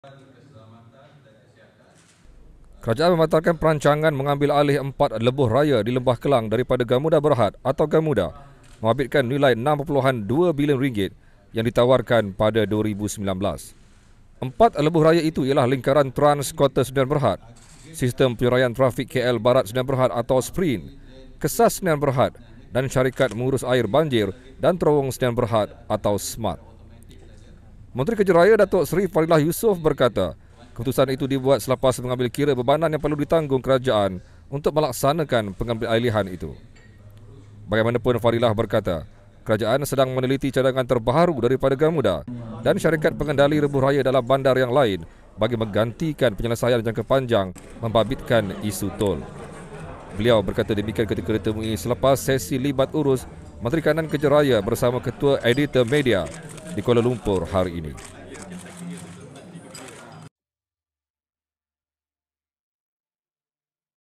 Kerajaan batal ambil alih empat lebuh raya
KUALA LUMPUR: Kerajaan membatalkan perancangan mengambil alih empat lebuh raya di Lembah Klang daripada Gamuda Berhad (Gamuda) membabitkan nilai RM6.2 bilion, yang ditawarkan pada 2019.

Empat lebuh raya terbabit ialah Lingkaran Trans Kota Sdn Bhd, Sistem Penyuraian Trafik KL Barat Sdn Bhd (SPRINT), KESAS Sdn Bhd dan Syarikat Mengurus Air Banjir dan Terowong Sdn Bhd (SMART).

Menteri Kerja Raya, Datuk Seri Fadillah Yusof, berkata keputusan itu dibuat selepas mengambil kira bebanan yang perlu ditanggung kerajaan untuk melaksanakan pengambilalihan itu.